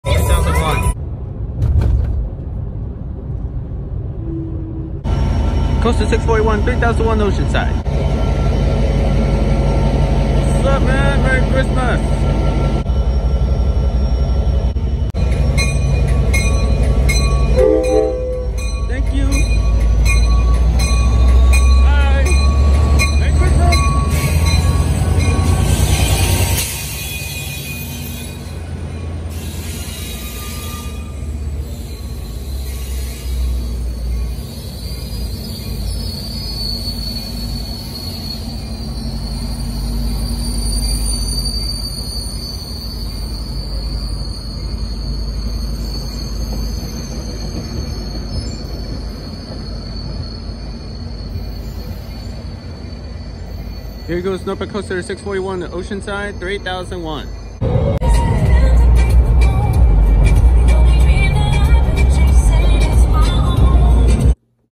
Ocbus Amtrak Sprinter And NCTD Coaster At San Clemente And Oceanside 12/21/22
San Clemente CA